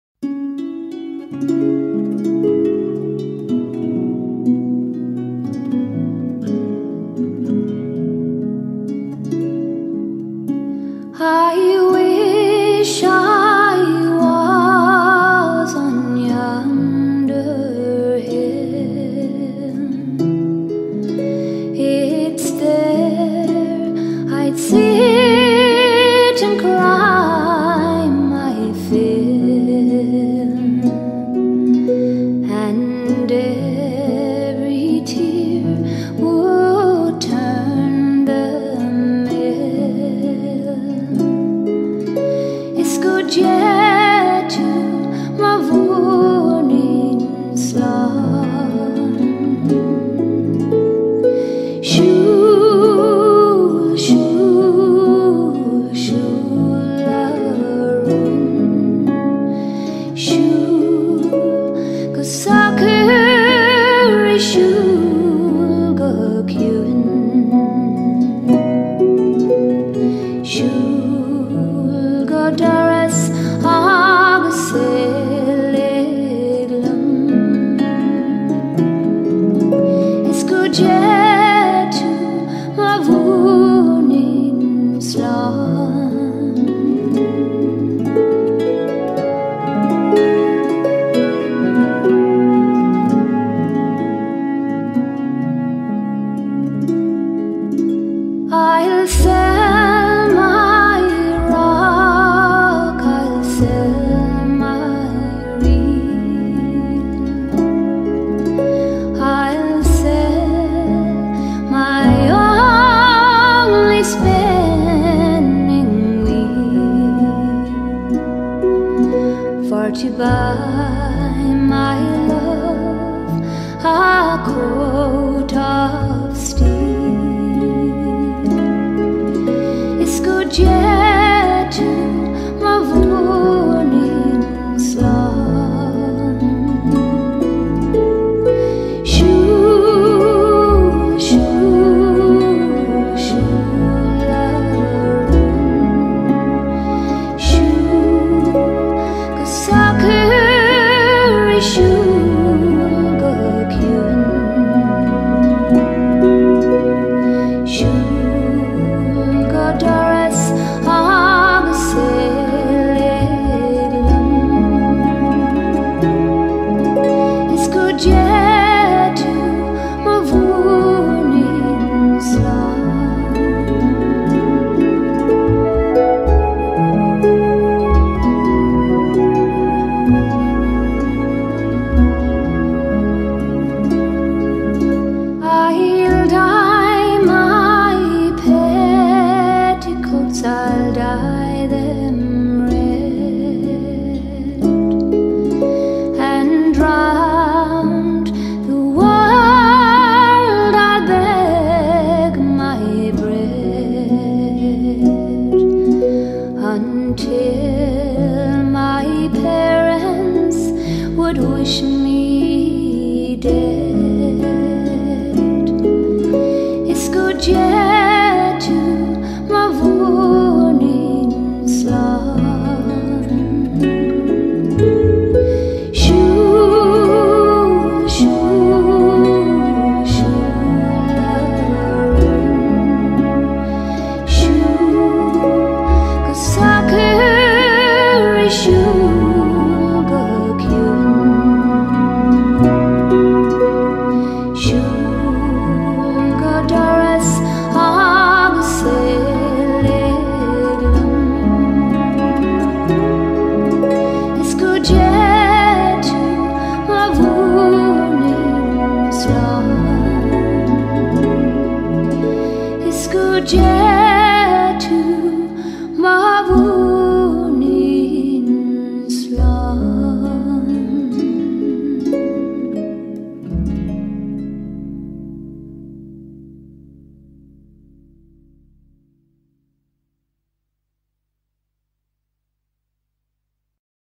Tina Mulrooney Cliffs of Moher musician
Tina Mulrooney is a celtic harpist & singer who performs regularly at the Cliffs of Moher in County Clare Ireland.  This video was shot at the Cliffs and shows both the wild beauty of the cliffs and the magical music that Tina creates.  She holds one of the busking licences for the Cliffs of Moher